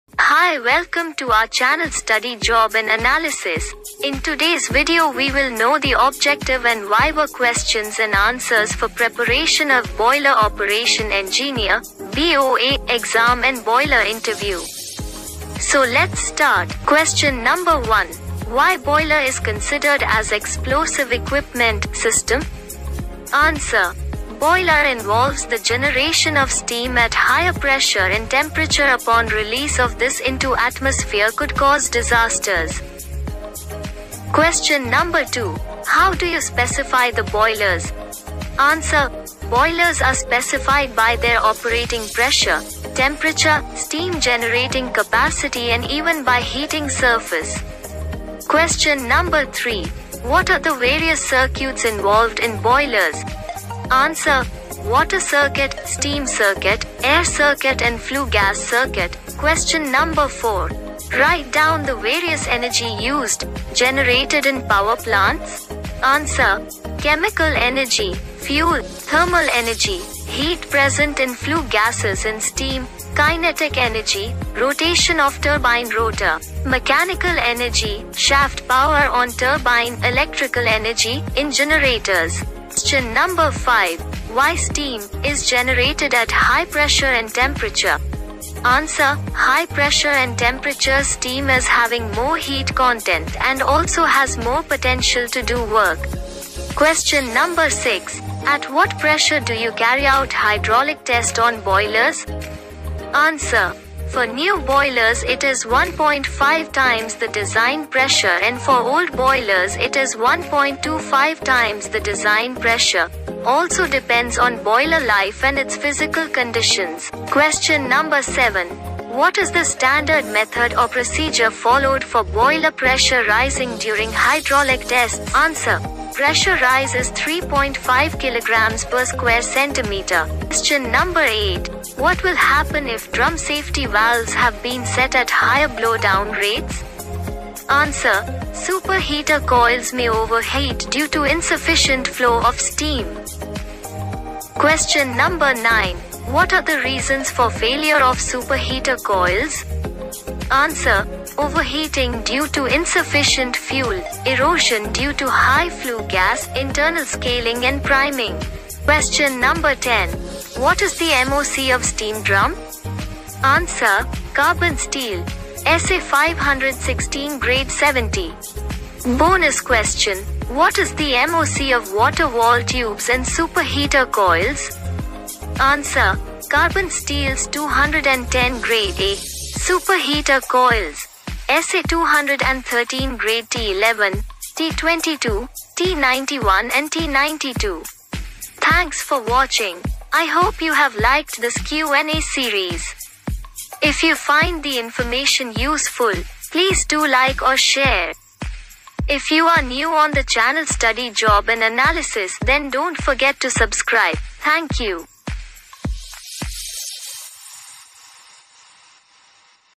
BOE interview Questions Answers | BOE exam preparation boe boe_exam part-2
bOE exam PREPARATION
boiler QUESTION AND ANSWER
BOE EXAMINATION
BOILER OPRATION ENGINEERING
top 10 BOE questions

QUESTION NO 2,-How do you specify the Boilers?

Question no3, -What are the various circuits involved in Boilers?

-Write down the various energy used / generated in power plants?

 5-Why steam, is generated at high pressure & temperature?

High pressure & temperatures steam is having more heat content & also has more potential to do
work.
 6-At what pressure do you carryout Hydraulic test on Boilers?

 7-What is the standard method or procedure followed for boiler pressure rising during hydraulic
tests?

8-What will happen if drum safety valves have been set at higher blow down rates?

QUESTION NO 9, -What are the reasons for failure of super heater coils?

Question no 10,What is the MOC of steam drum?Bonus Question, What is the MOC of water wall tubes & super heater coils?